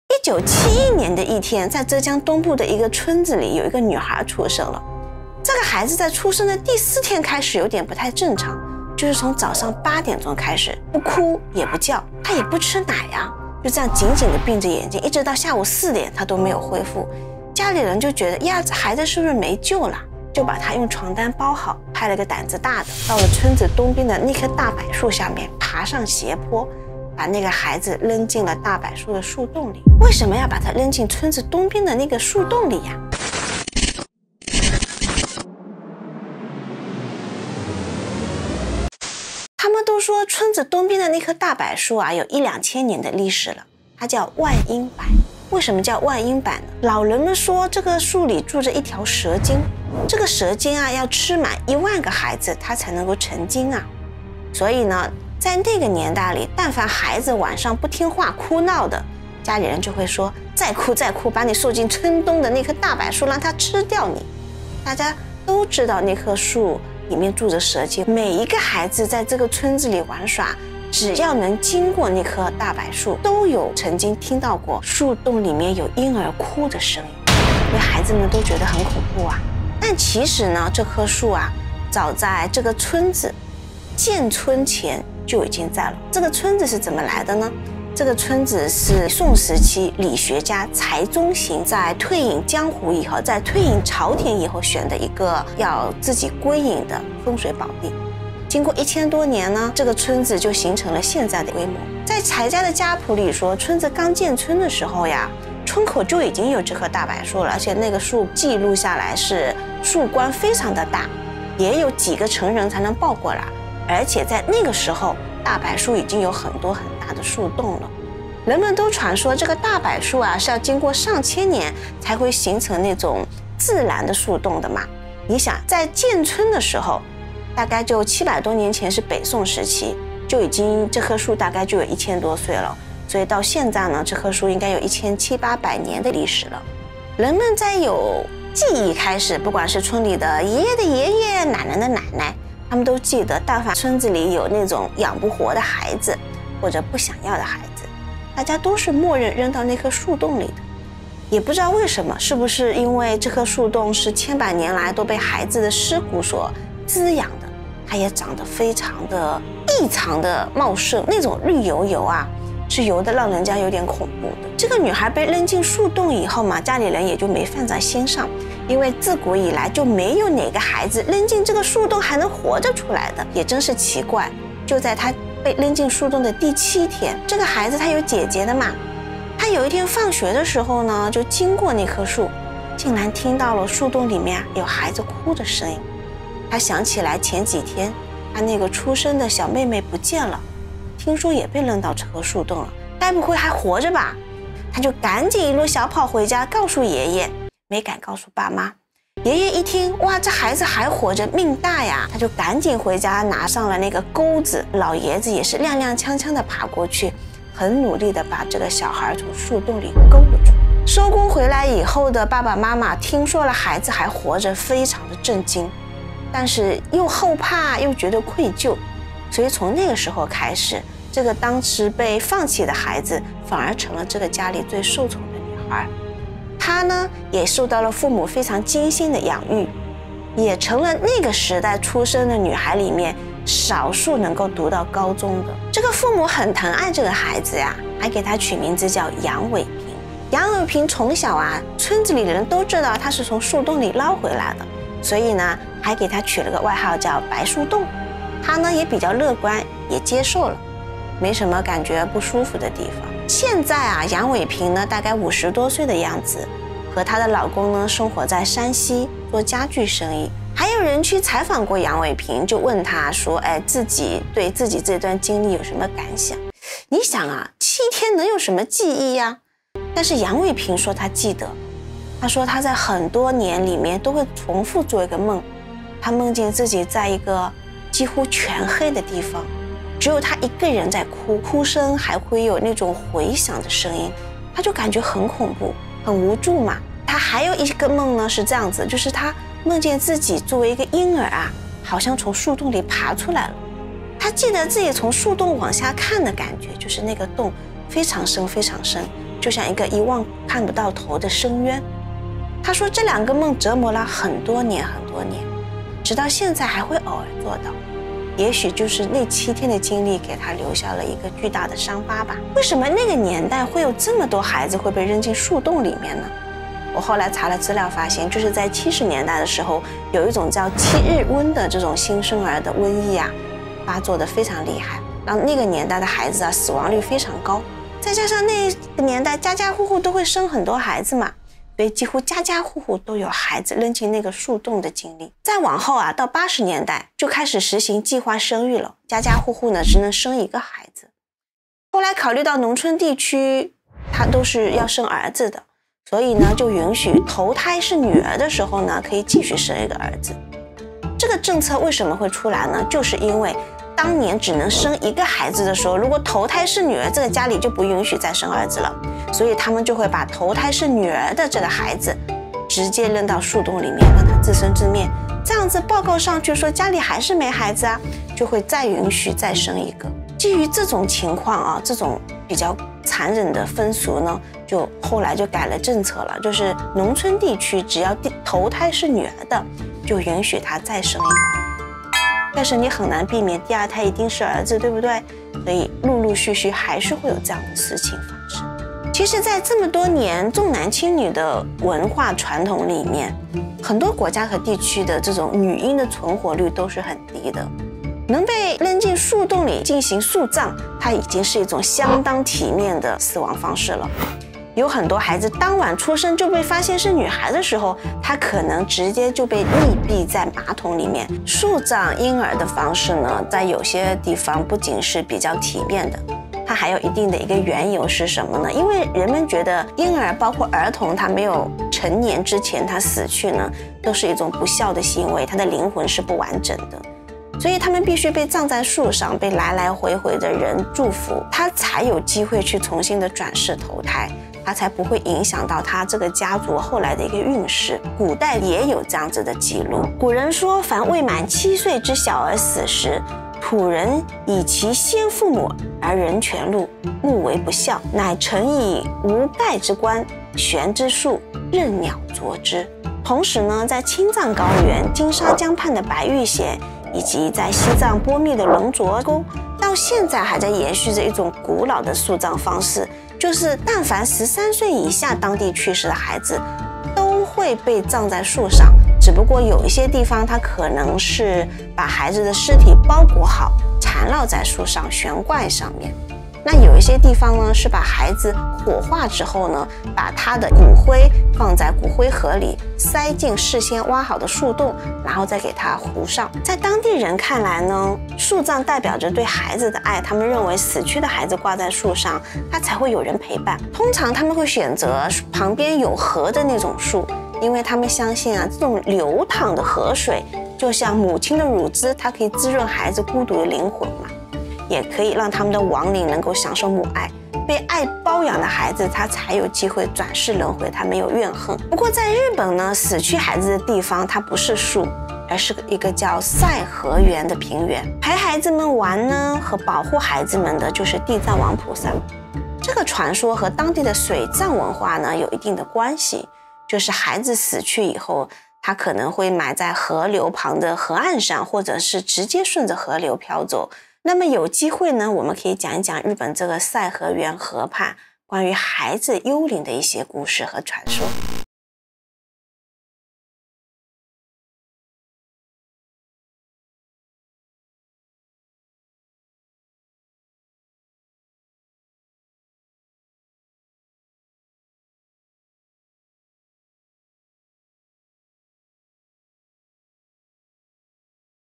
千年古樹竟是萬嬰墳場?從浙江萬嬰柏到西藏嬰童天葬樹，10分鐘了解駭人的樹葬風俗和樹洞生還者50年後的回憶。揭開重男輕女時代下女嬰的悲慘命運的祕密
從浙江一個村莊的萬嬰柏講起，數千年的樹葬傳統和一些不爲人知的可怕觀念。

- 章節
- 00:00 震驚！浙江村莊驚現千年"嬰兒吞噬樹"
- 02:31 樹洞生還者50年後首度揭秘驚人記憶
- 04:20 70年代"七日瘟"與萬嬰柏的血腥真相驚人關聯
- 05:52 80年代農村女嬰的悲慘命運
- 07:05 華人世界神秘樹葬文化大揭秘
- 08:24 中國、日本的兒童亡靈安息地